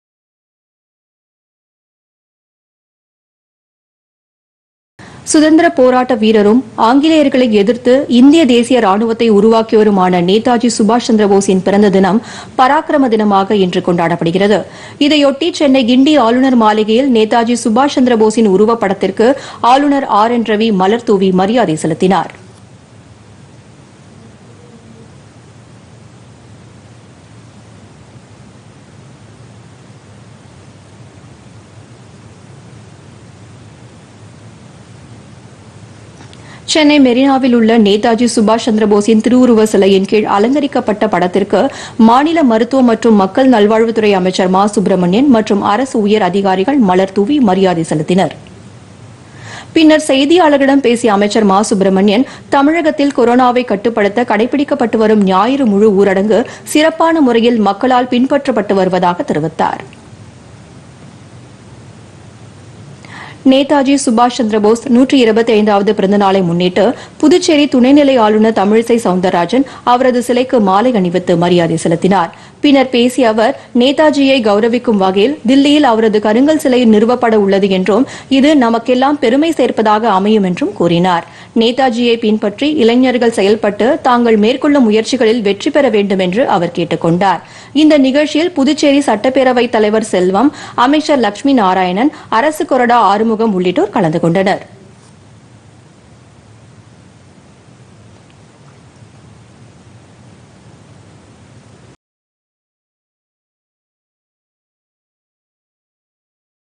நேதாஜி பிறந்த தினம்: ஆளுநர்கள், அமைச்சர்கள் மரியாதைPodhigaiTamilNews
நேதாஜி சுபாஷ் சந்திர போஸ் பிறந்தநாள் விழா  -  தமிழக ஆளுநர் ஆர் என் ரவி, புதுச்சேரி துணை நிலை ஆளுநர் தமிழிசை சவுந்த்ராஜன், அமைச்சர்கள் மாலை அணிவித்து மரியாதை.